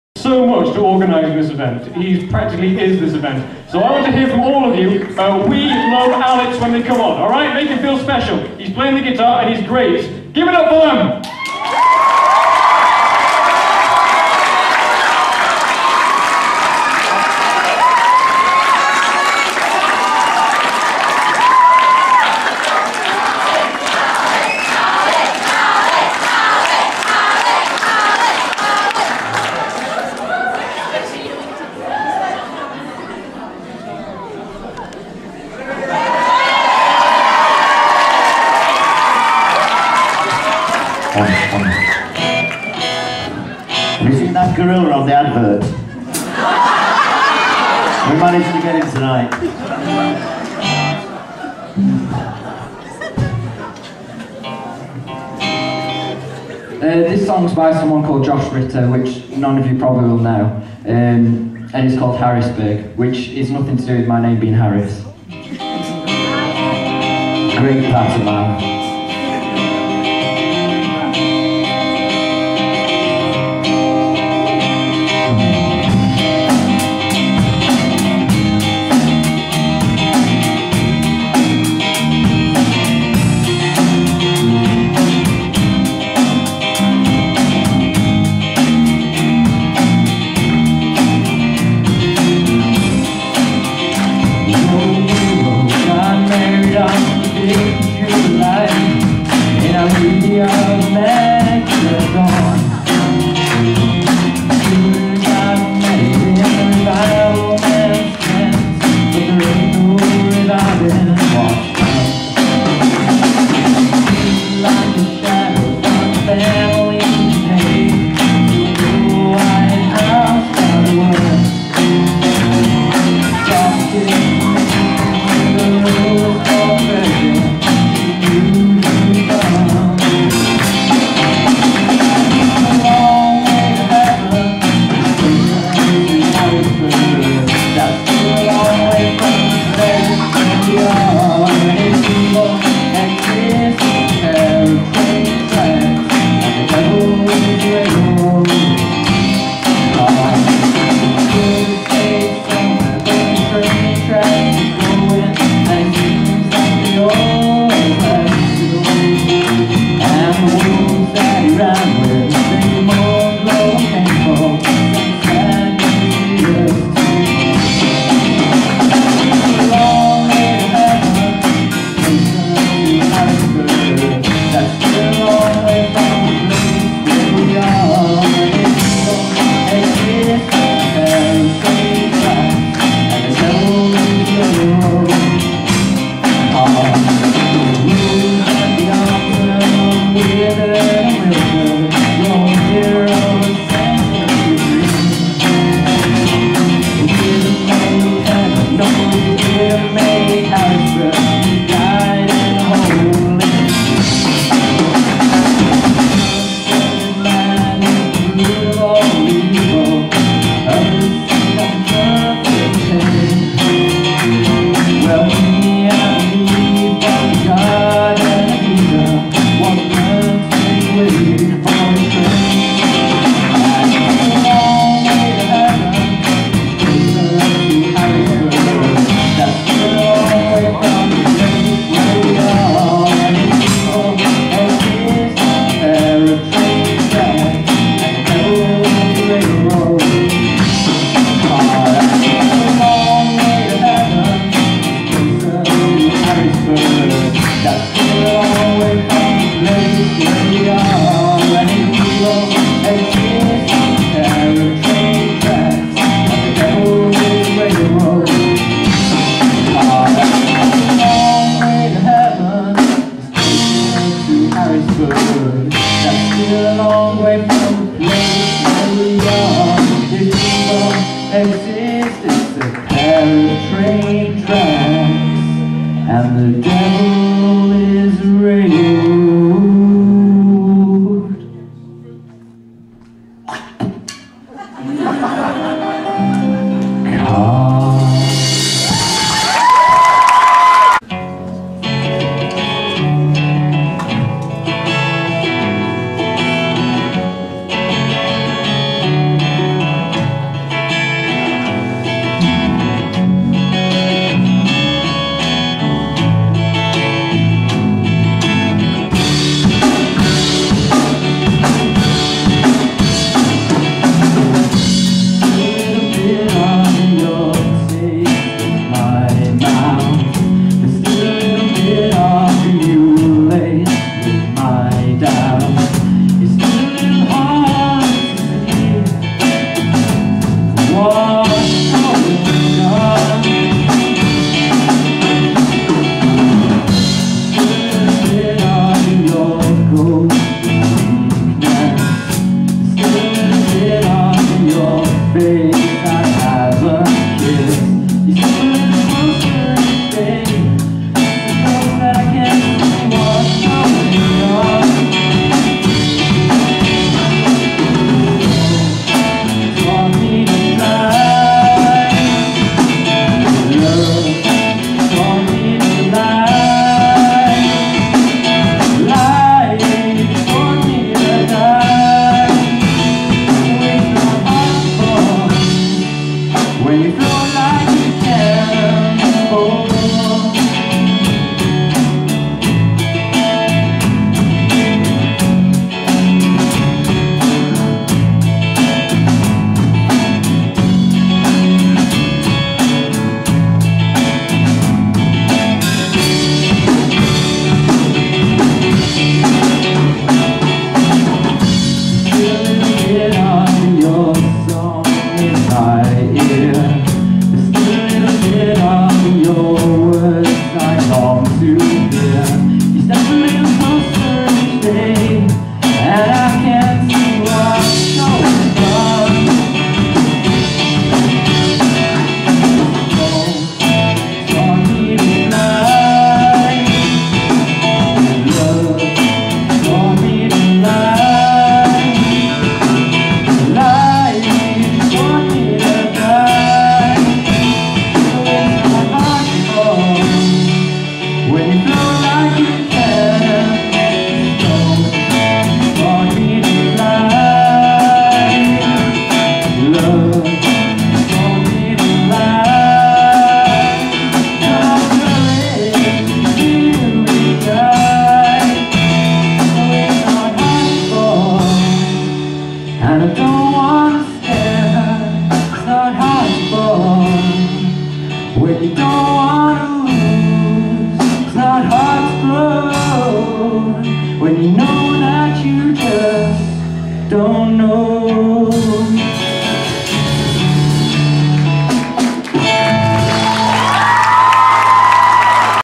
Music relief 08 part 1
Music relief Concert at Dinnington Comprehensive School.

Charity Concert for Macmillan